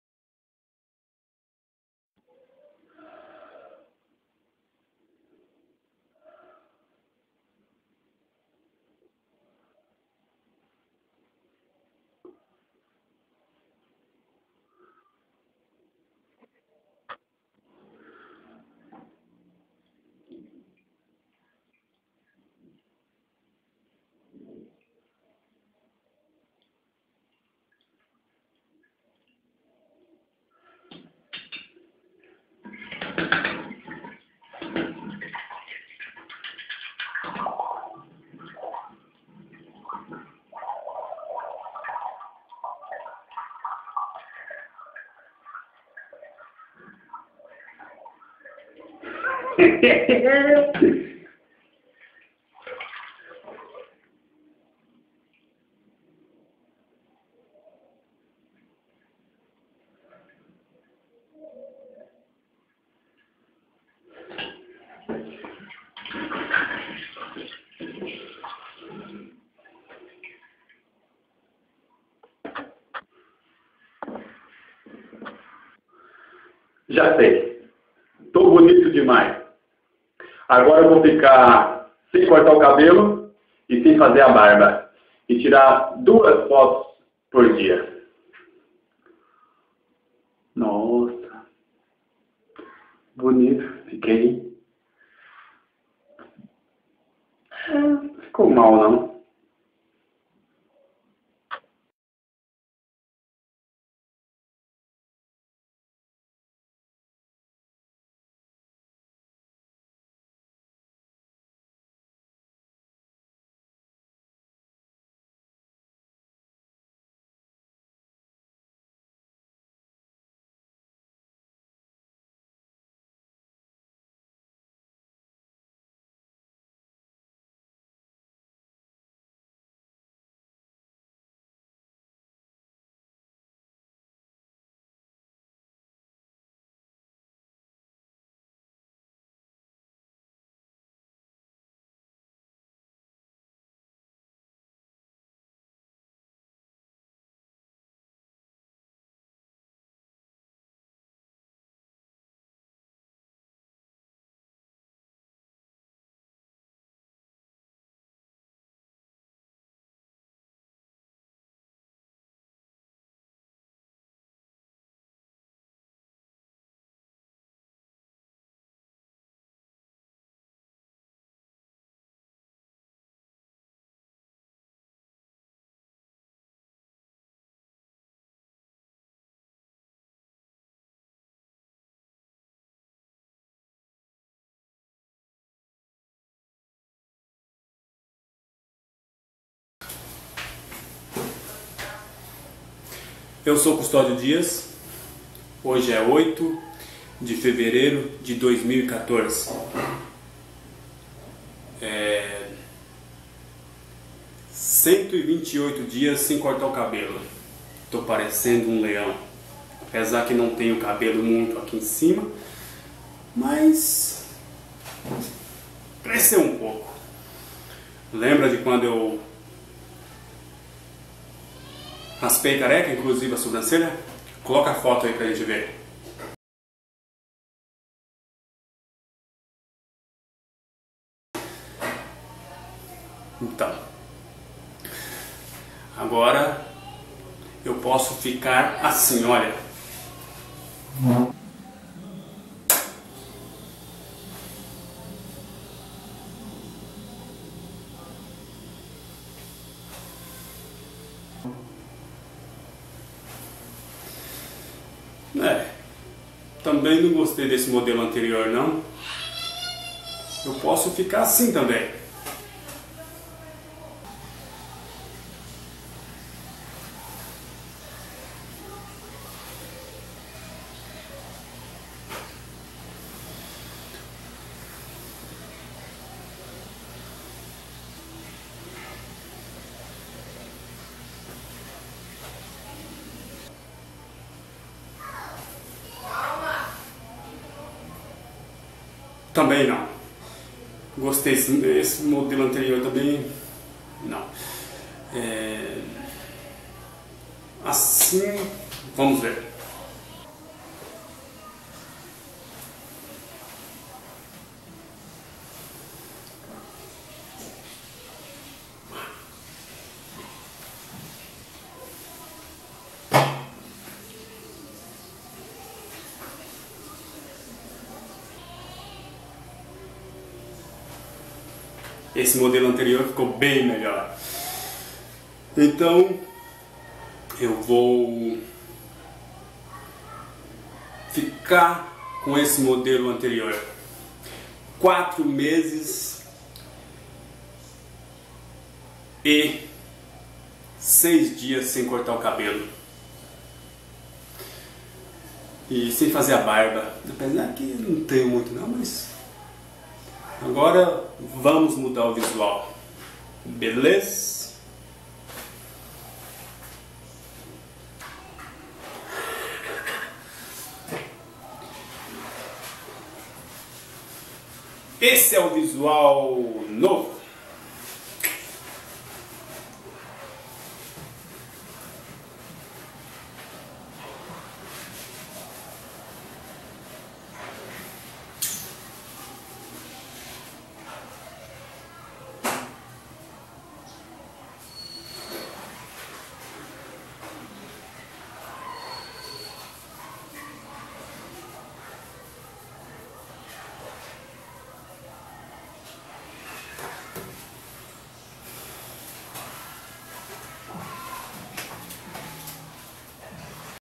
SELFIES 4 MESES E SEIS DIAS SEM CORTAR O CABELO
128 DIAS SEM CORTAR O CABELO E SEM FAZER A BARBA | CUSTODIO DIAS